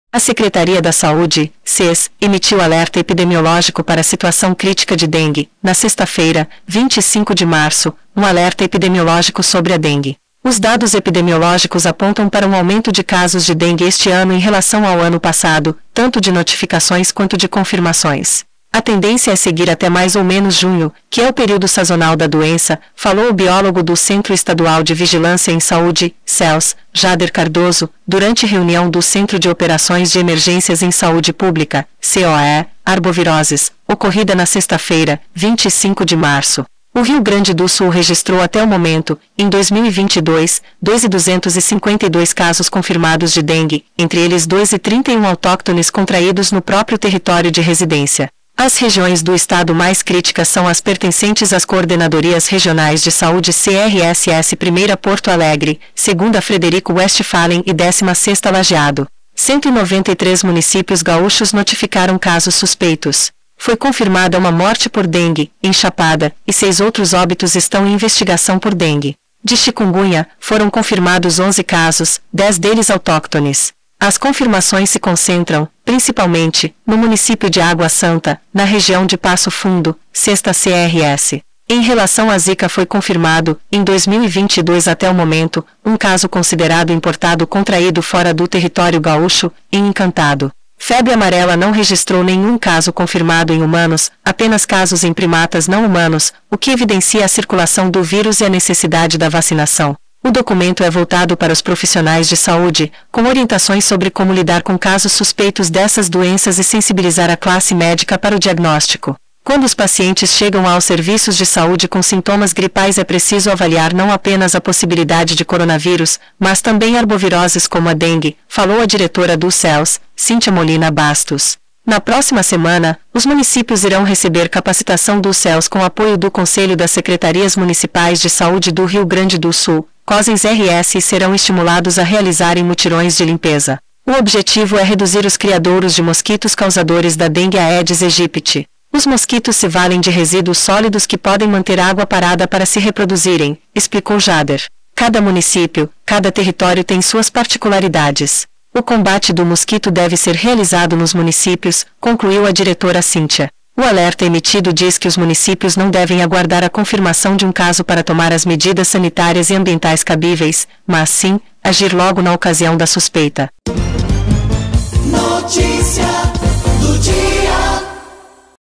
DENGUE - Secretaria Estadual da Saúde emite alerta epidemiológico para situação crítica de dengue
A Secretaria da Saúde (SES) emitiu nesta sexta-feira (25/03) um alerta epidemiológico sobre a dengue. “Os dados epidemiológicos apontam para um aumento de casos de dengue este ano em relação ao ano passado, tanto de notificações quanto de confirmações. A tendência é seguir até mais ou menos junho, que é o período sazonal da doença”, falou o biólogo do Centro Estadual de Vigilância em Saúde (Cevs), Jáder Cardoso, durante reunião do Centro de Operações de Emergências em Saúde Pública (COE) Arboviroses, ocorrida na sexta-feira (25/03).

O Rio Grande do Sul registrou até o momento, em 2022, 2.252 casos confirmados de dengue, entre eles 2.031 autóctones (contraídos no próprio território de residência). As regiões do Estado mais críticas são as pertencentes às Coordenadorias Regionais de Saúde (CRSs) 1ª (Porto Alegre), 2ª (Frederico Westphalen) e 16ª (Lajeado). 193 municípios gaúchos notificaram casos suspeitos. Foi confirmada uma morte por dengue, em Chapada, e seis outros óbitos estão em investigação por dengue.

De chikungunya, foram confirmados 11 casos, 10 deles autóctones. As confirmações se concentram, principalmente, no município de Água Santa, na região de Passo Fundo, 6ª CRS. Em relação à zika foi confirmado, em 2022 até o momento, um caso considerado importado (contraído fora do território gaúcho), em Encantado. Febre amarela não registrou nenhum caso confirmado em humanos, apenas casos em primatas não-humanos, o que evidencia a circulação do vírus e a necessidade da vacinação.

O documento é voltado para os profissionais de saúde, com orientações sobre como lidar com casos suspeitos dessas doenças e sensibilizar a classe médica para o diagnóstico. “Quando os pacientes chegam aos serviços de saúde com sintomas gripais é preciso avaliar não apenas a possibilidade de coronavírus, mas também arboviroses como a dengue”, falou a diretora do Cevs, Cynthia Molina Bastos.

Na próxima semana, os municípios irão receber capacitação do Cevs com apoio do Conselho das Secretarias Municipais de Saúde do Rio Grande do Sul (Cosems/RS) e serão estimulados a realizarem mutirões de limpeza. O objetivo é reduzir os criadouros de mosquitos causadores da dengue Aedes aegypti. “Os mosquitos se valem de resíduos sólidos que podem manter água parada para se reproduzirem”, explicou Jáder. “Cada município, cada território tem suas particularidades. O combate do mosquito deve ser realizado nos municípios”, concluiu a diretora Cynthia. O alerta emitido diz que os municípios não devem aguardar a confirmação de um caso para tomar as medidas sanitárias e ambientais cabíveis, mas sim, agir logo na ocasião da suspeita.